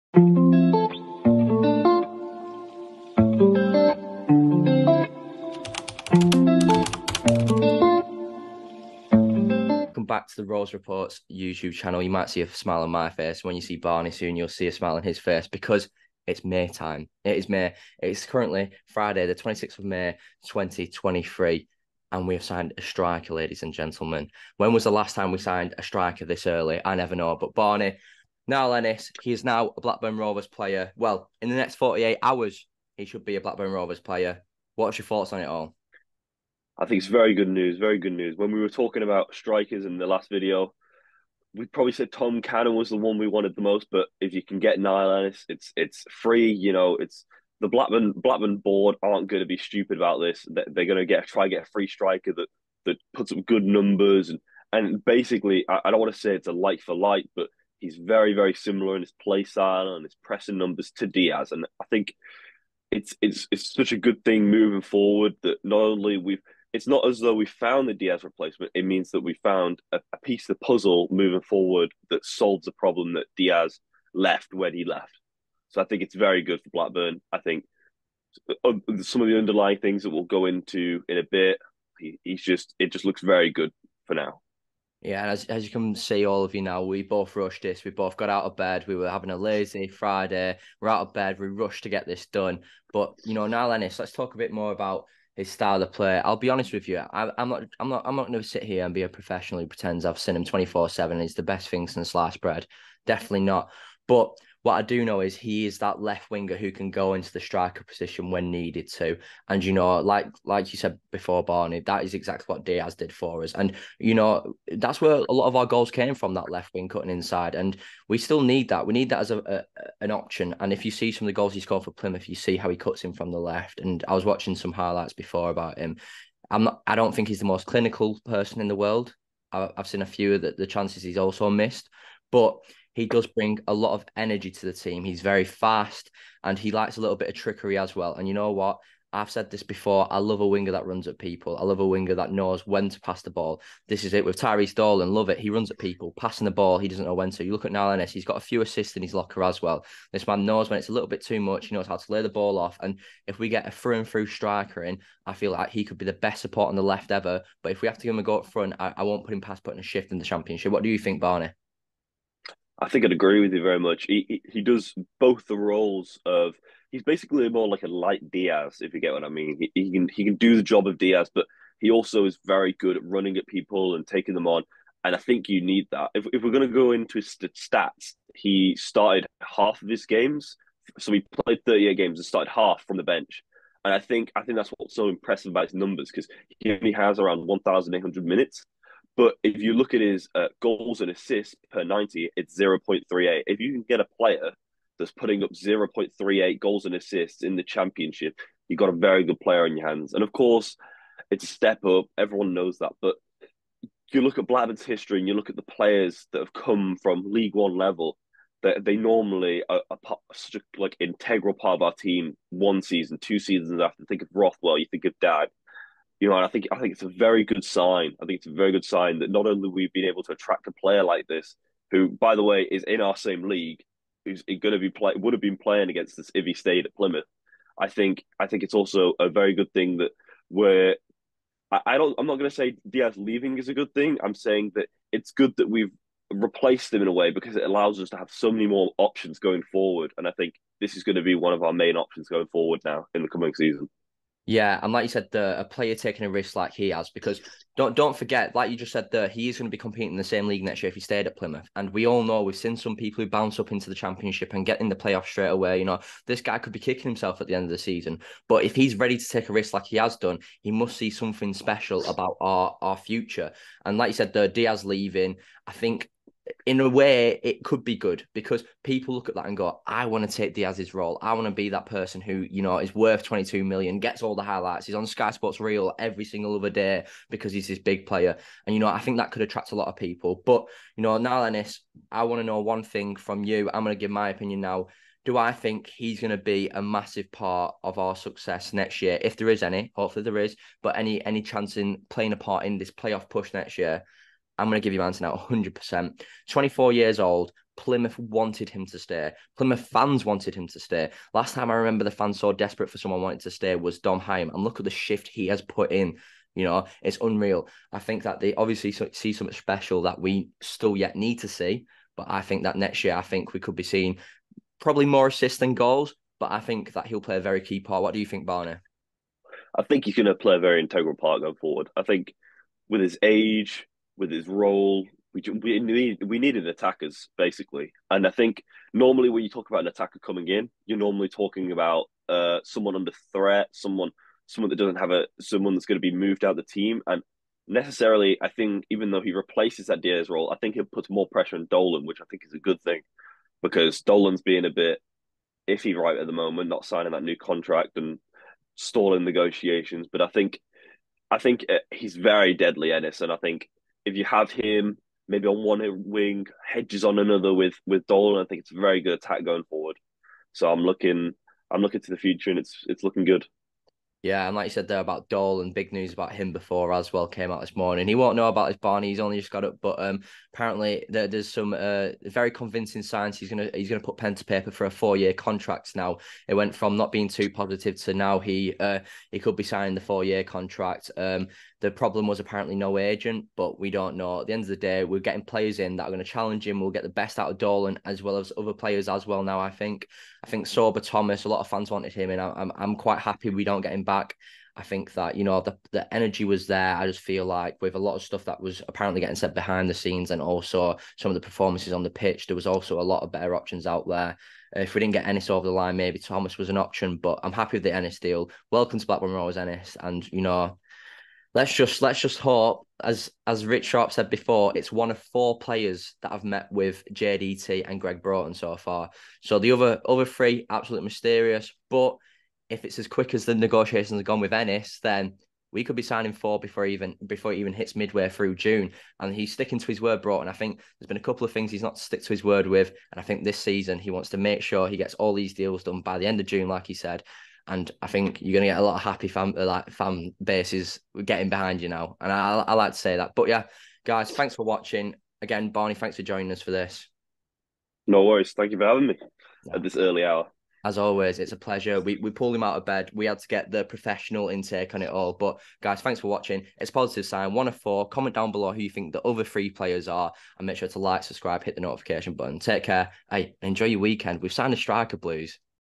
NIALL ENNIS SIGNS FOR BLACKBURN ROVERS - ROVERS SIGN A STRIKER!🤯
Sit down with Matt and Barney as they tell you all you need to know about Blackburn's new boy Niall Ennis!✅